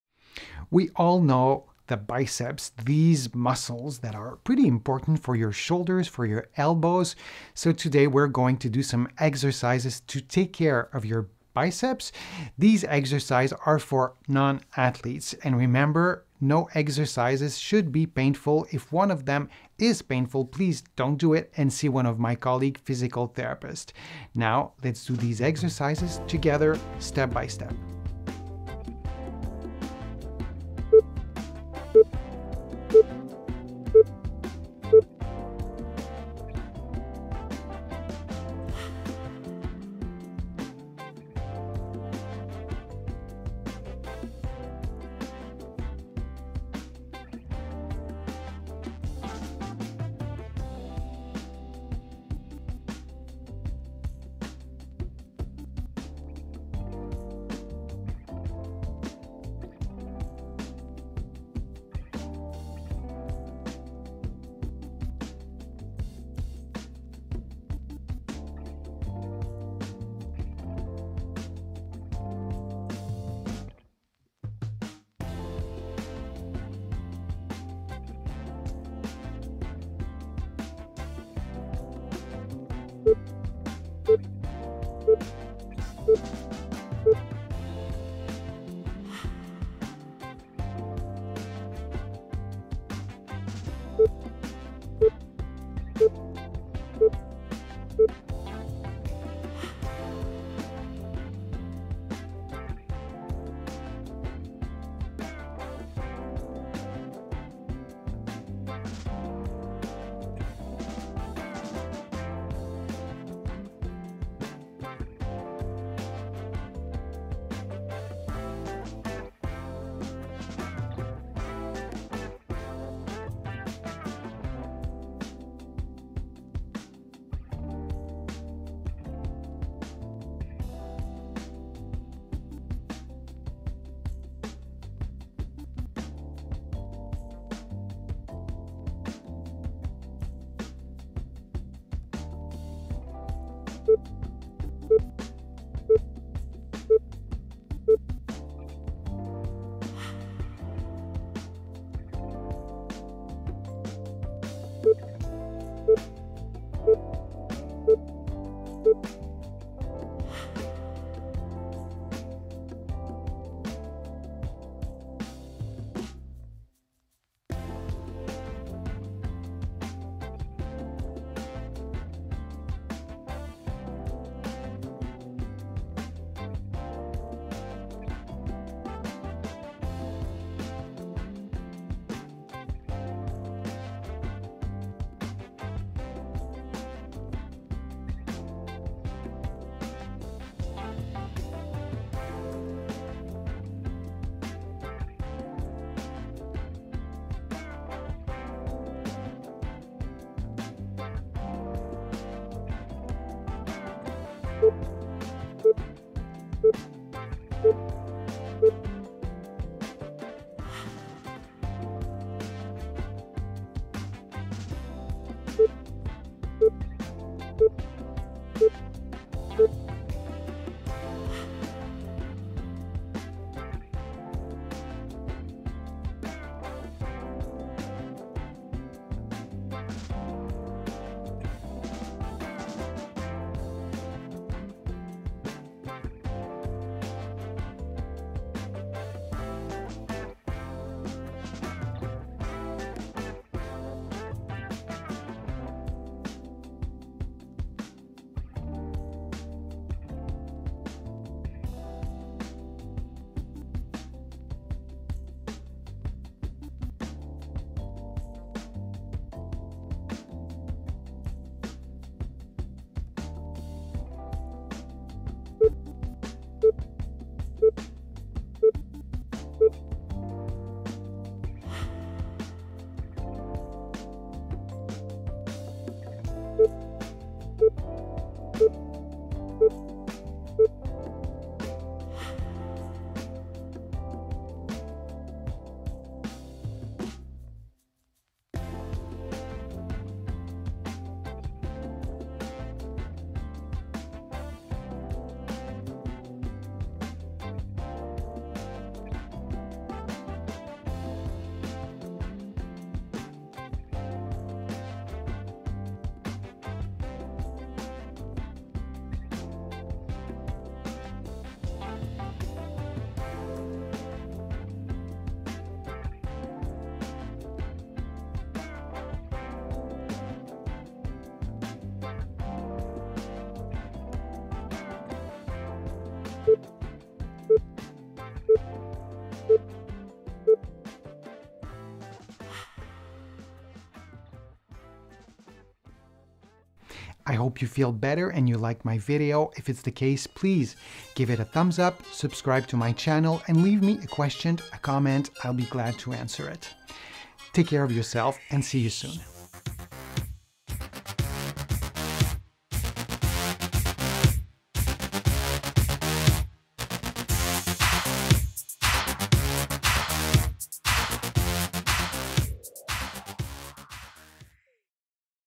Biceps exercices for non-athletes
How to improve biceps strength?  With these made easy exercises. So easy to do at home !

This 9 minutes workout will improve your arms strength and will help you to keep or increase your endurance!

* for non-athletes

Take care.

Denis.